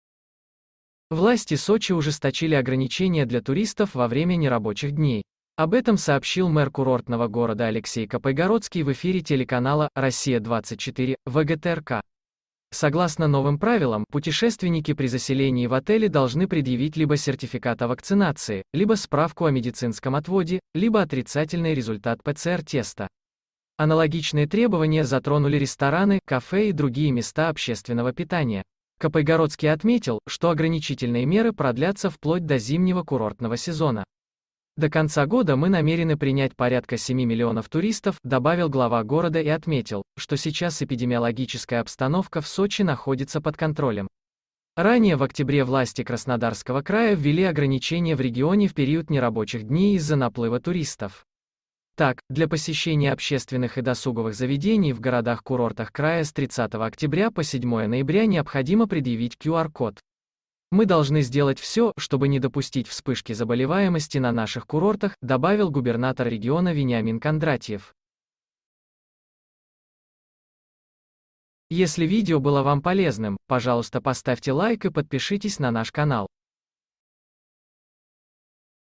В Сочи ужесточили правила заселения в отели для туристов
Власти Сочи ужесточили ограничения для туристов на ноябрьские праздники. Согласно новым правилам, путешественники при заселении в гостиницы должны предъявить либо сертификат о вакцинации, либо справку о медицинском отводе, либо отрицательный результат ПЦР-теста. Ограничения продлятся до зимнего курортного сезона.